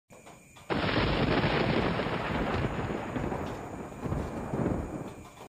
Impressive Lightning Strike | 11-05-19 (Philippines)
This was my lightning chase from our backyard. The strong-slammer of a lightning occurred before
thunderstorm had arrived into our place, the origin of the place of the thunderstorm which was located to southern cities of Metro Manila while it poses a threat as it heading in to Cavite.This was one of a severe-type of a thunderstorm about that we've got a thunderstorm for the month of November in year 2019.

Taken Date/Time: November 05, 2019 | 22:18 / 10:18 PM | Philippine Local Time (PST/PHT)

Captured Location: Malagasang 1-G, Imus, Cavite, PH

This footage of lightning chase is genuinely owned by Charlton Gio V. Prepeña